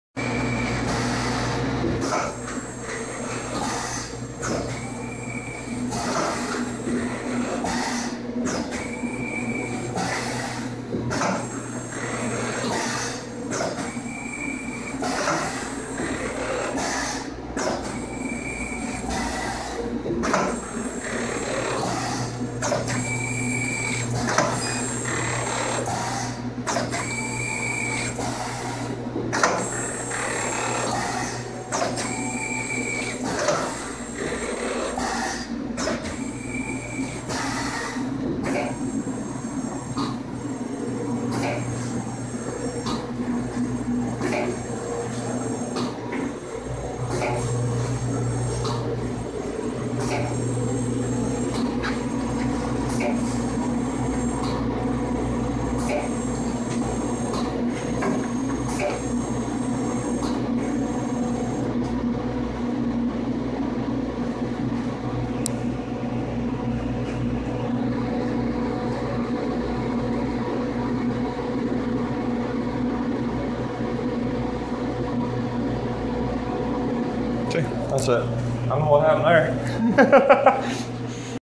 FUG ( This is to our fans) YOU Rock!
Just a snip of us printing up our winning design in the National Screen Printing design contest! This video is for our fans! We are printing with everything we purchased